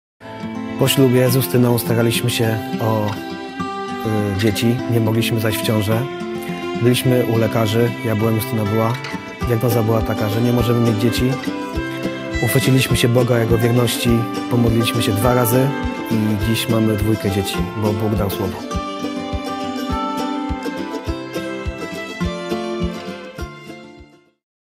Justyna i Kamil
Justyna i Kamil mają dwóch synów - bo On dał słowo.
W świecie zawiedzionych nadziei i złamanych obietnic jest ktoś, kto zawsze dotrzymuje słowa.
Posłuchaj historii ludzi, którzy Mu zaufali.